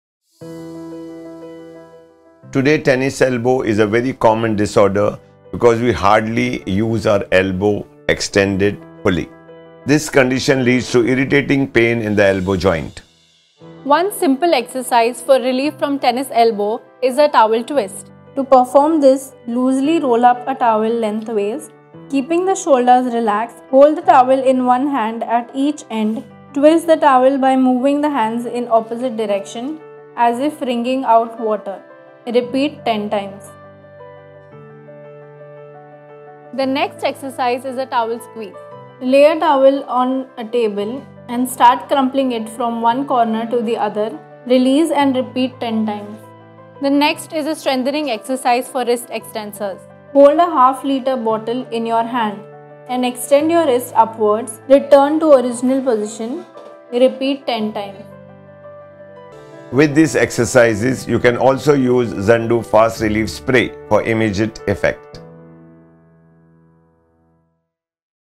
Tennis Elbow Pain Management
Tennis Elbow Pain Management Strategies. In this video, we delve into effective techniques and exercises to manage and reduce tennis elbow pain. Stay in the game with less pain!